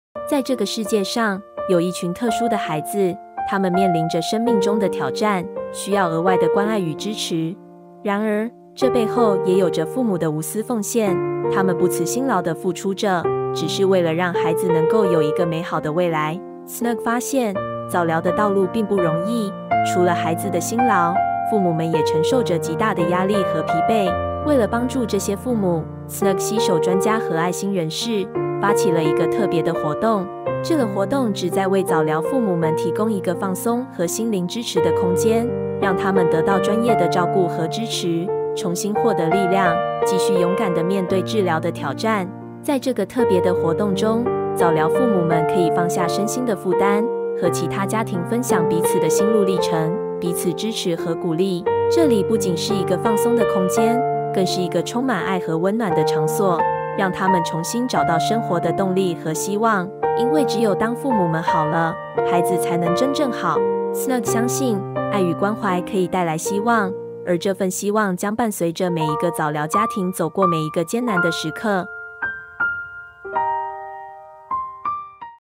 【給早療家庭的媽咪們放鬆時光】早療關懷活動-媽咪鬆一下💕
sNug相信愛與關懷可以帶來希望，而這份希望將伴隨著每一個早療家庭，走過每一個艱難的時刻

2018年的11月，因為一位馬蹄足幼童媽媽的許願，sNug給足呵護進入了這群【足特別】孩子的世界！
透過與這些早療媽媽談話的過程中，sNug發現早療家庭除了孩子辛苦，媽媽或主要照顧者的身心也極度疲累，很需要心靈和身體的放鬆紓壓，唯有媽媽或主要照顧者好，孩子才會好🫶🫶🫶
因此，sNug特別邀請到同樣是早療媽媽的全悅關懷協會創會長、美加德聯合診所院長(骨科醫師)，中醫師及復健師、護理師，一群充滿愛心的志工朋友，一同發起了【媽咪鬆一下】早療家庭關懷活動！創造一個讓辛苦的馬蹄足早療媽媽們有一個放鬆的時光，補充身心能量，支持媽咪們繼續前行。

【媽咪鬆一下】早療家庭關懷活動無須報名費用，活動期間也將由志工全程照顧孩子，帶孩子進行遊戲互動，我們提供媽咪下午茶交流時間、中、西醫和物理治療師現場教學與諮詢，為媽媽們進行筋膜放鬆。希望藉由活動，能讓媽咪們全方位達到身心放鬆，同時幫助媽媽們學習如何自我防護等衛教。

🙌歡迎早療家庭的媽媽或主要照顧者報名參加🙌
🙌也歡迎邀請更多願意提供場地或擔任愛心志工的朋友一起加入我們🙌